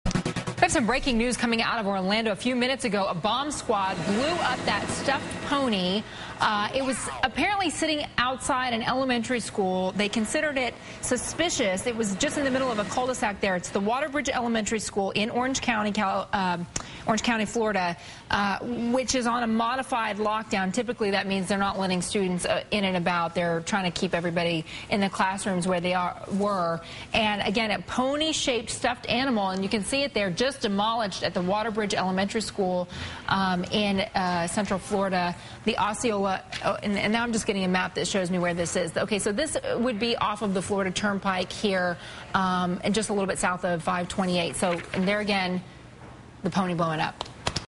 exploding_pony.flv
Stuffed pony blown up at Orlando school:
Initially, the Bomb Squad taunted the pony with a robot, but it stoically refused to react to the provocation. So they blew it up.